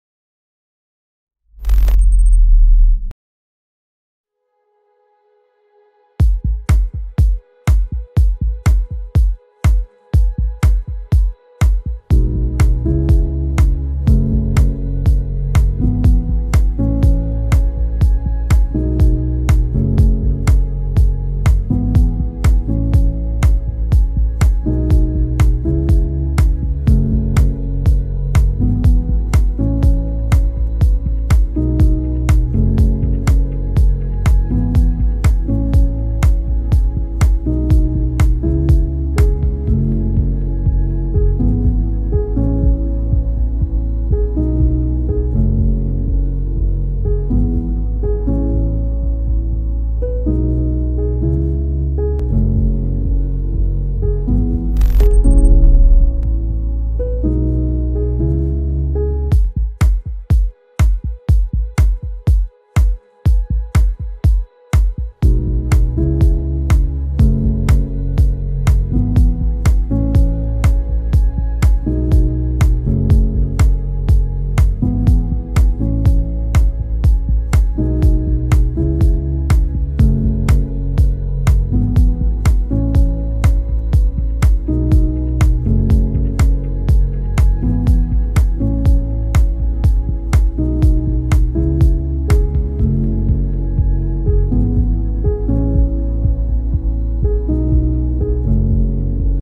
MG x DS ART CAR - Spray painting the new MG ZS London Design Week 2019
MG x DS ART CAR